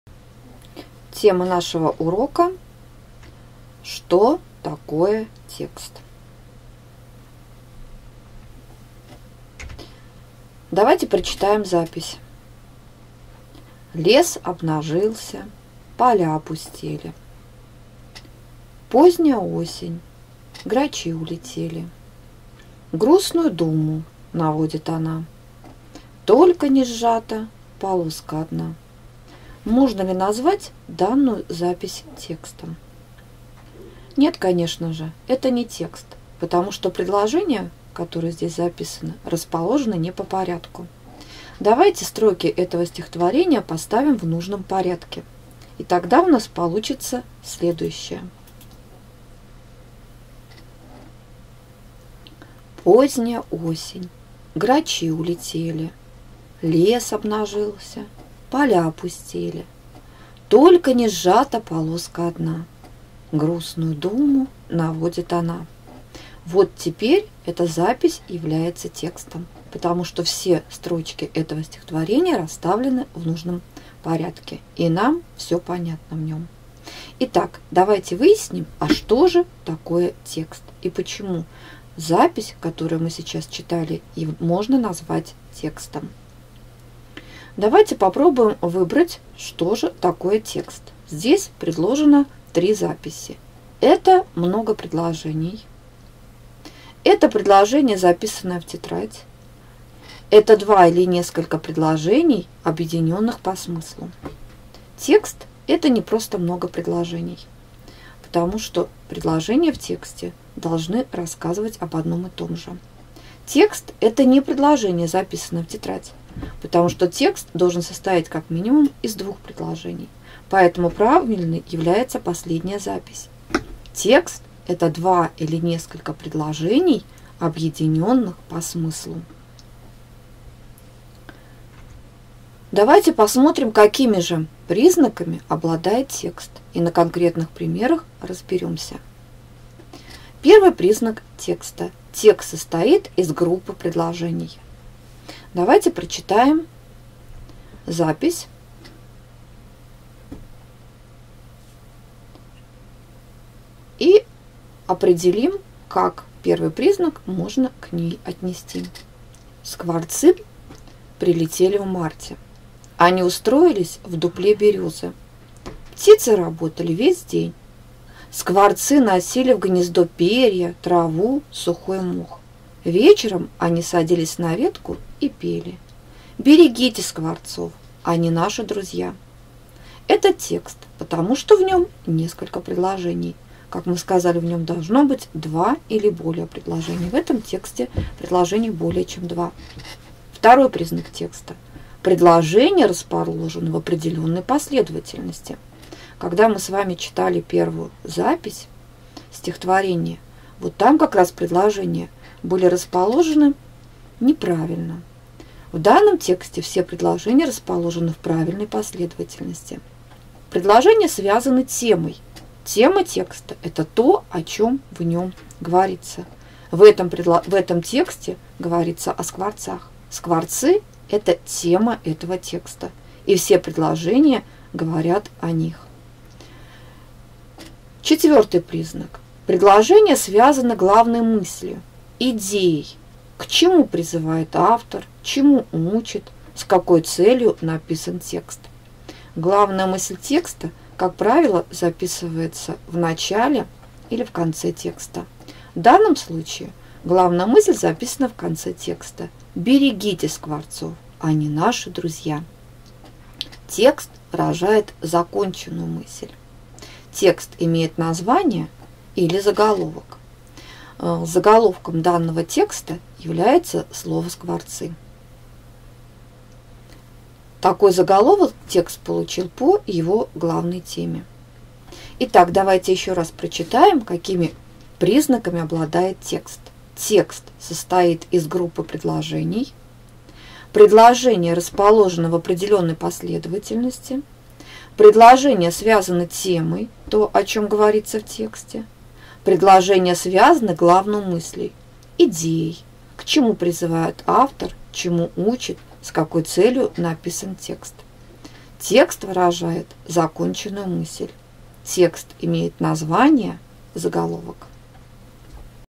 Русский язык 3 класс. (Канакина) Тема 3: Что такое текст
Видео урок по русскому языку «Что такое текст?» направлен на систематизацию знаний ребенка о признаках текста. Посмотрев видео он сможет дать определение понятию «текст» и безошибочно отличать текст от набора предложений.